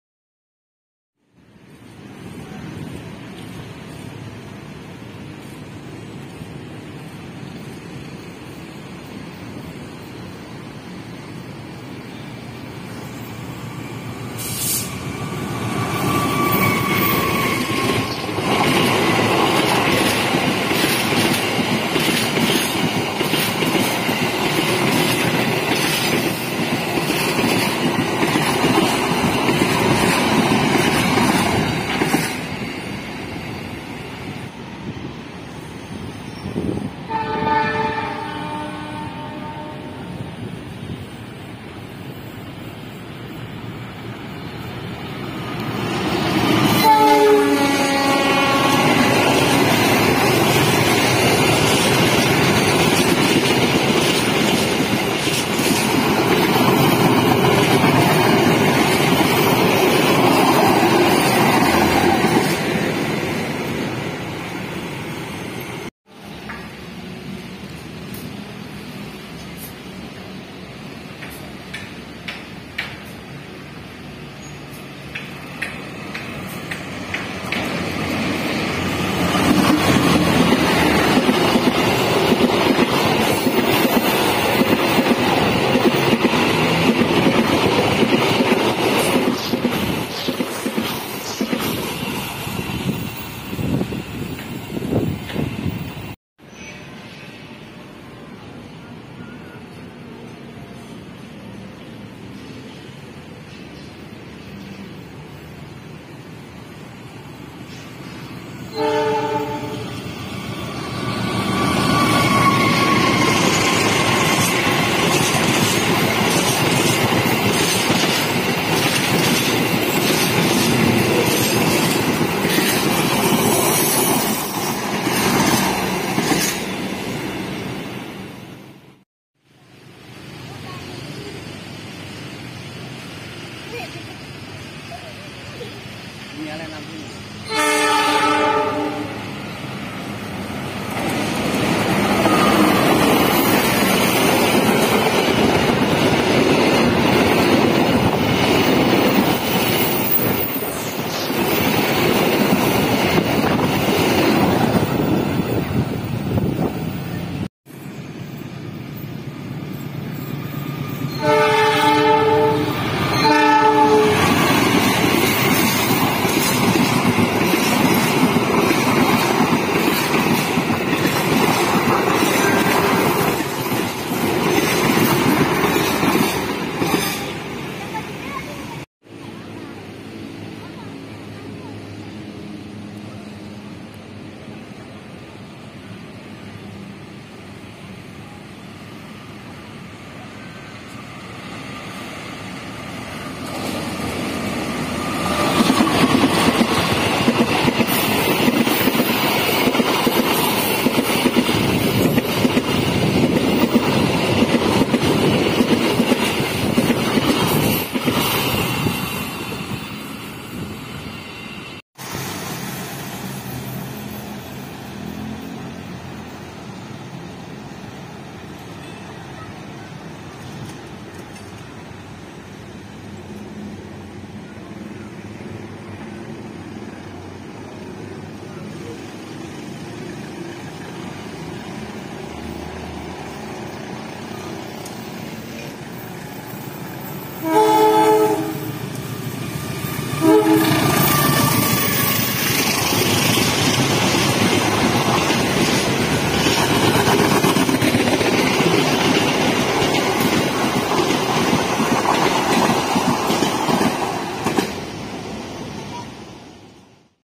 Hunting Kereta Api di Pagi menjelang Siang Hari di Jembatan Flyover Kranji Special HUT KAI 78
KAI 78周年記念クランジ特別高架橋での午前中、正午前の列車狩り